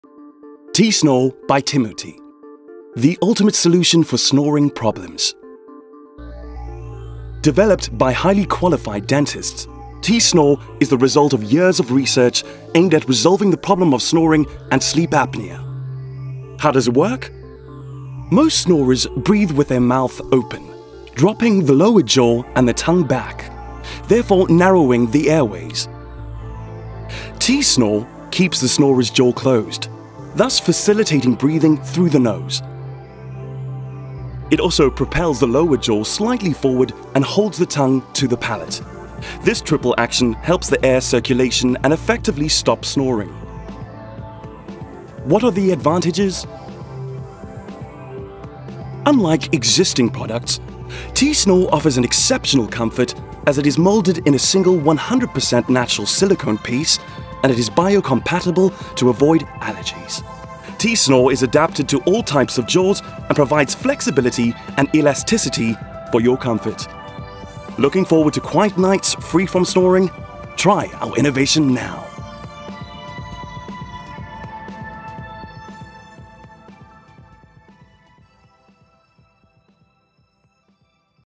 STOP SNORING - T-SNORE
T-SNORE is a medical device developed and patented by dentist surgeons in order to reduce snoring, sleep apnea and teeth grinding from the first nights